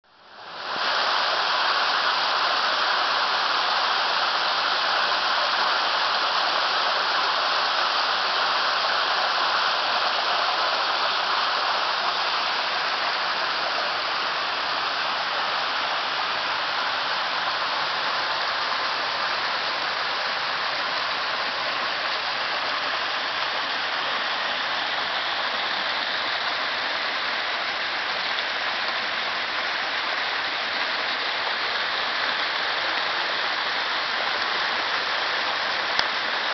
08 08 09 Palobbia Braone loc. Piazze-Scalassone presa enel e relativo irrisorio rilascio M.D.V.
la Captazione per uso idroelettrico  in località Piazze-Scalassone sul torrente Palobbia di Braone, è spesso totalmente chiusa impedendo il normale deflusso di acqua nel torrente Palobbia con conseguente moria di trote Fario, dopo varie denunce si è provveduto a rilasciare un irrisorio DMV.
Una presa si...ma per il culo